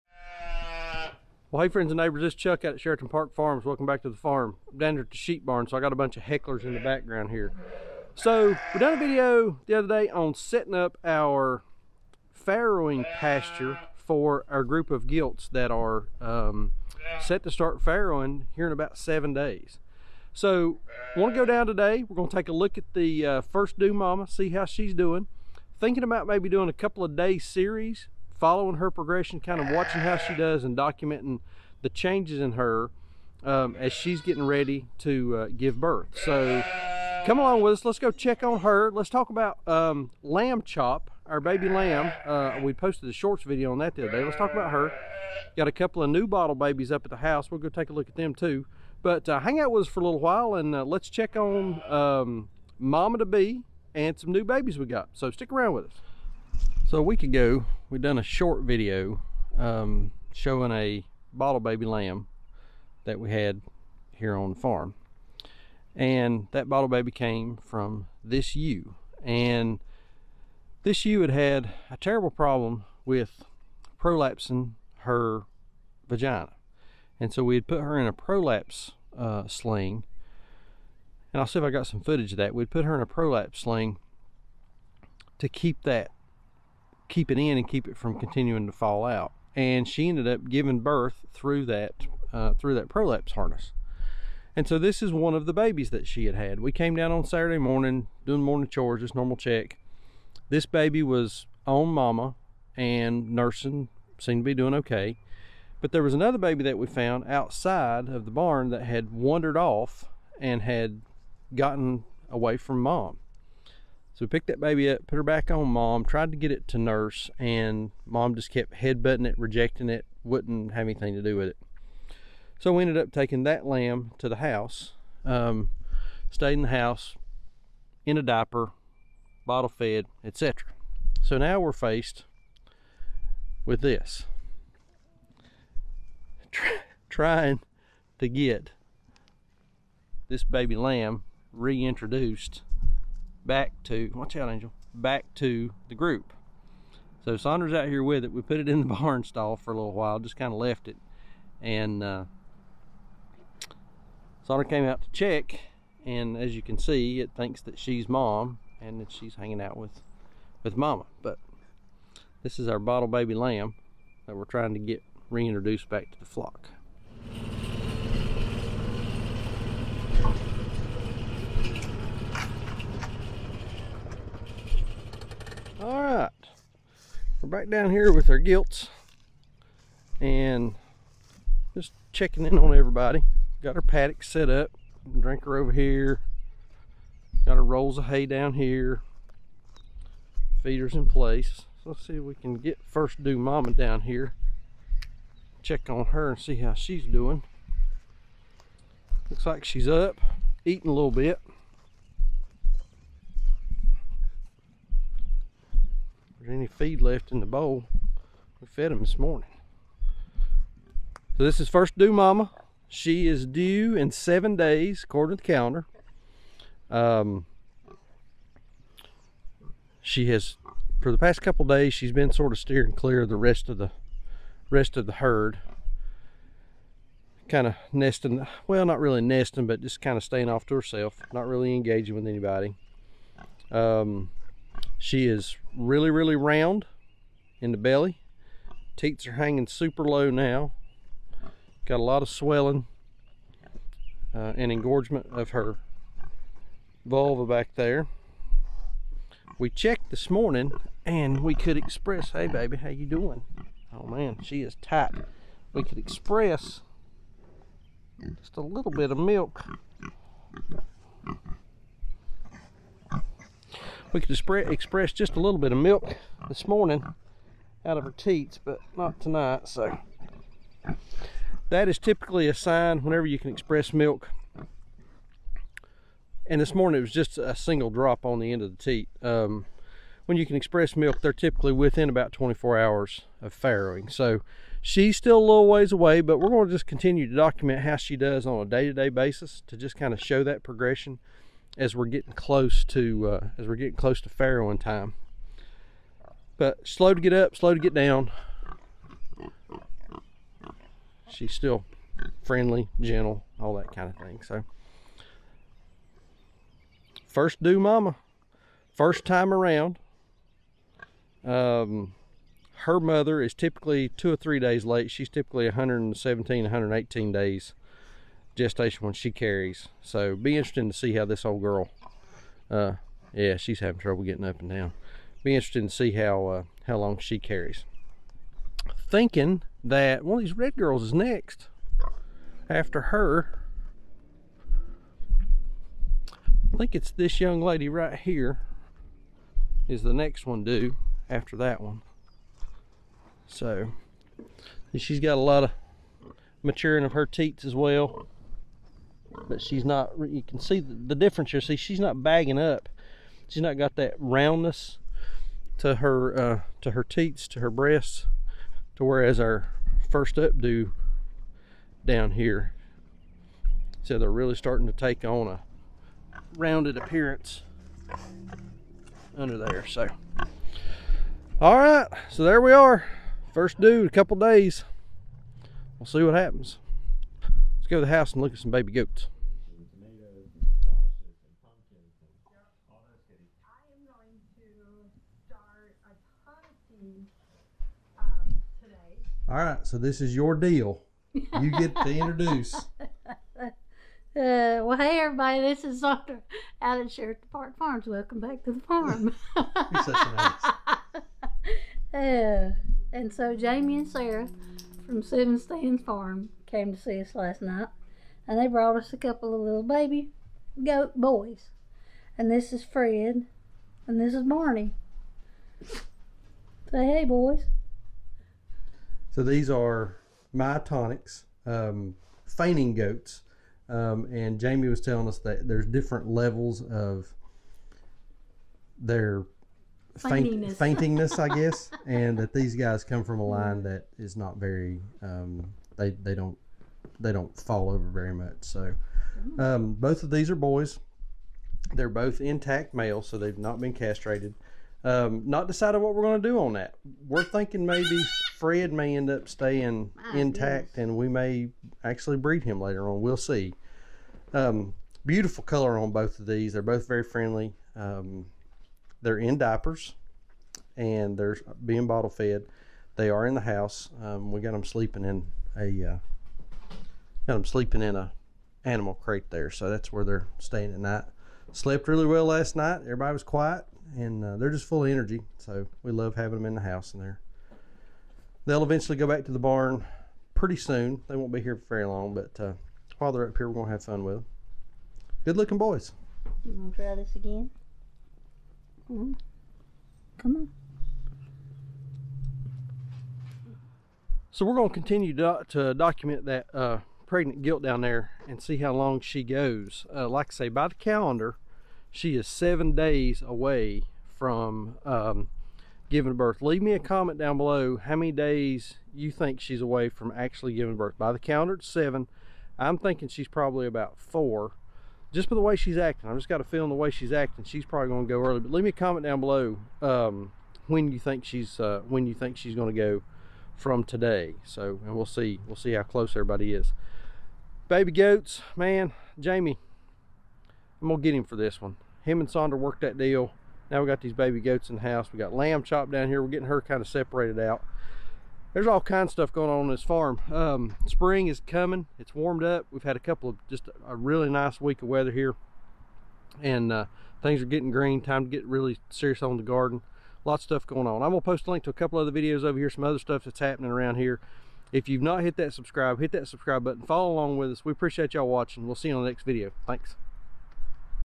Pastured Pigs - WHEN Will She Give Birth
We have 8 pregnant pigs and the first one is set to give birth sometime in the next week.  Come along as we document one of the gilts final week of pregnancy, monitor the changes she goes through and hopefully catch the blessed event!

Oh, and there are baby goats on the farm!  Cuteness overload.